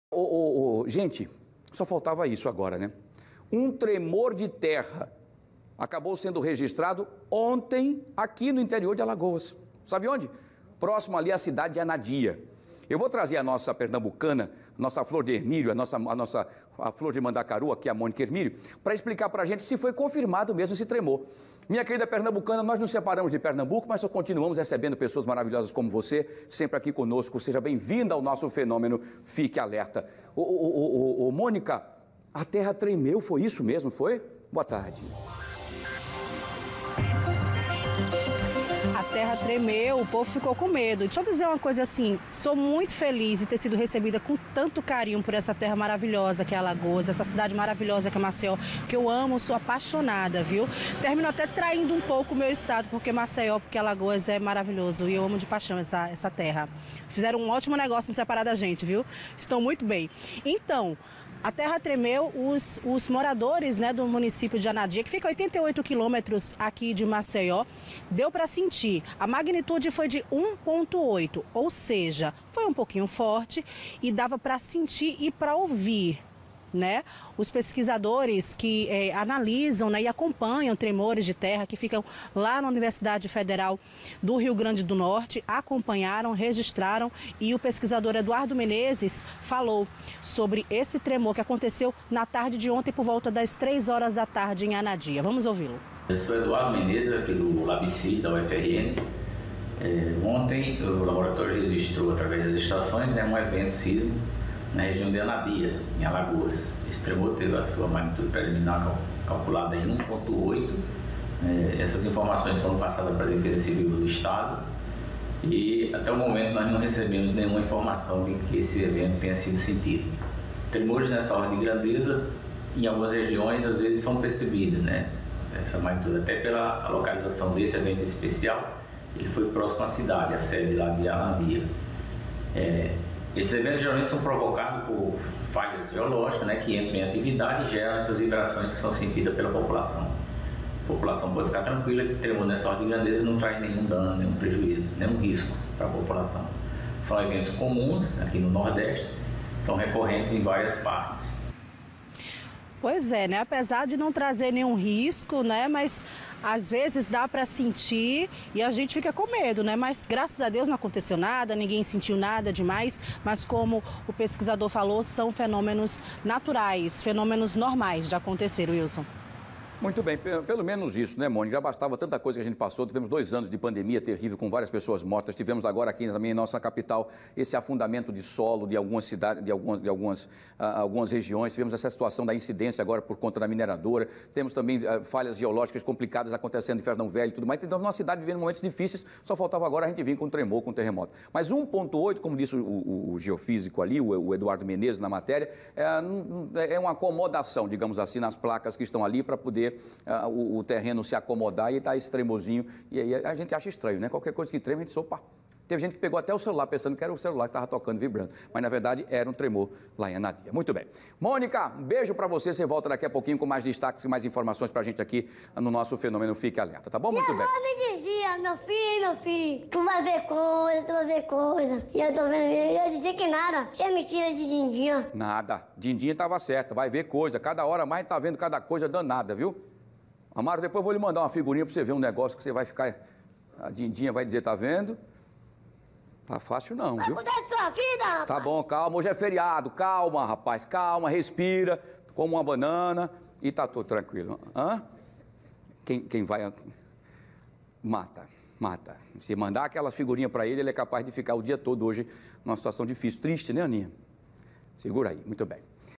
Tremor de terra de terra foi registrado no interior de Alagoas, próximo à cidade de Anadia
Acompanhe mais detalhes sobre a situação, na reportagem de Monica Ermírio.